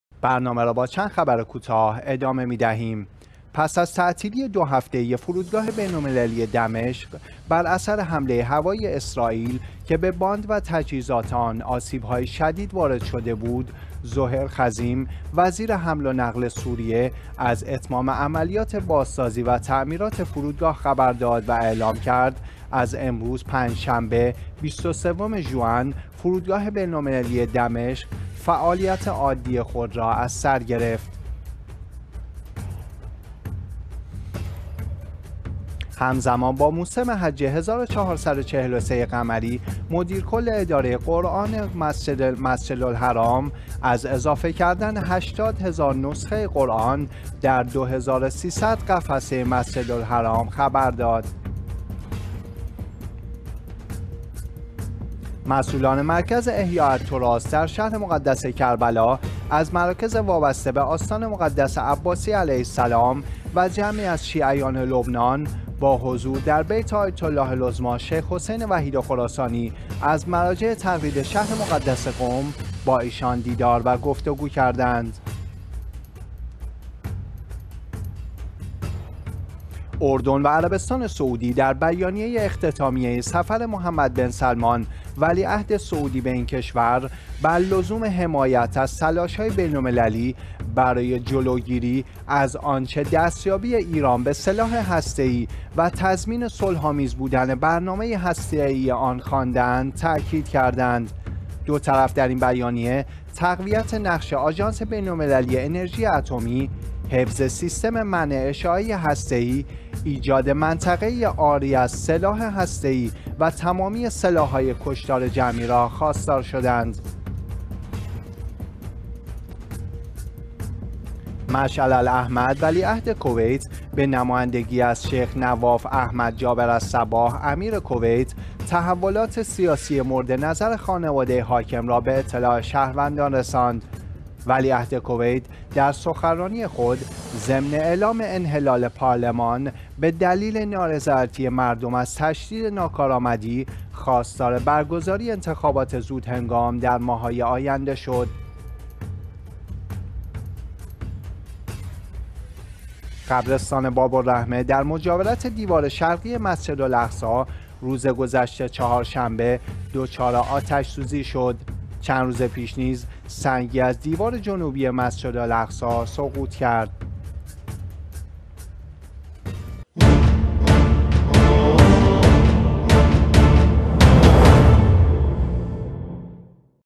نگاهی به چند خبر کوتاه: 2 تیر 1401
1)بازگشت فرودگاه بین‌المللی دمشق به حالت عادی
2)عرضه ۸۰ هزار نسخه جدید قرآن در مسجد الحرام
3)دیدار مسئولان مرکز احیاء التراث کربلا با آیت الله العظمی وحید خراسانی
4)صدور بیانیه مشترک عربستان و اردن علیه ایران
5)انحلال پارلمان کویت توسط ولیعهد
6)آتش‌سوزی در قبرستان باب الرحمه در مجاورت مسجد الاقصی